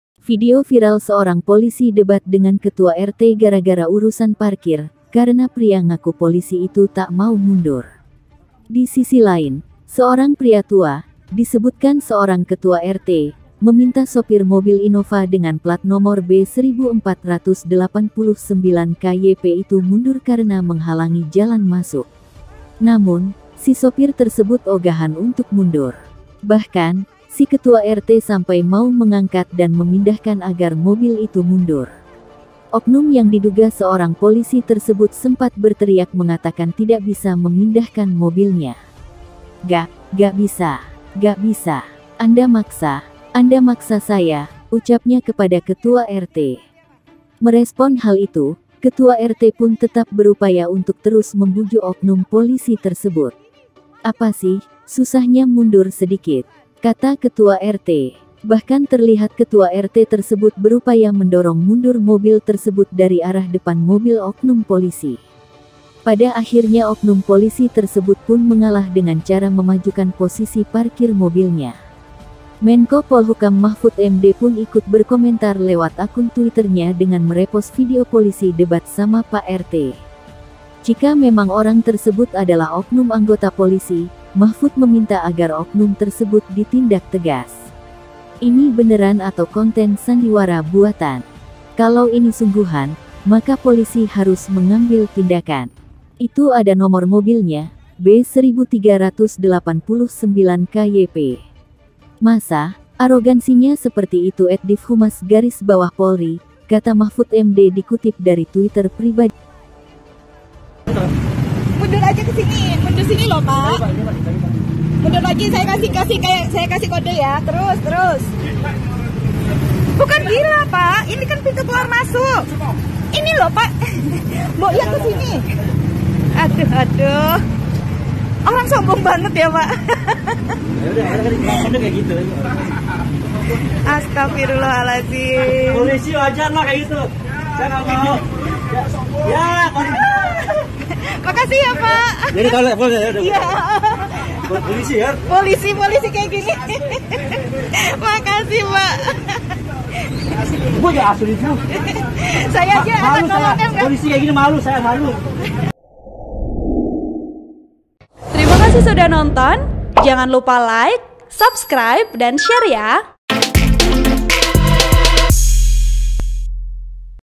Mahfud MD Sampai Jengkel, Polisi Ogah Parkir Mundur lalu Debat dengan Pak RT yang Renta
TRIBUNCIREBON.COM - Video viral seorang polisi debat dengan ketua RT gara-gara urusan parkir, karena pria ngaku polisi itu tak mau mundur.

Di sisi lain, seorang pria tua, disebutkan seorang ketua RT, meminta sopir mobil Innova dengan plat nomor B 1489 KYP itu mundur karena menghalangi jalan masuk.

Namun, si sopir tersebut ogahan untuk mundur. Bahkan, si ketua RT sampai mau mengangkat dan memindahkan agar mobil itu mundur.

Oknum yang diduga seorang polisi tersebut sempat berteriak mengatakan tidak bisa memindahkan mobilnya.

Video Editor: Fillaraz Mutia Sabrilla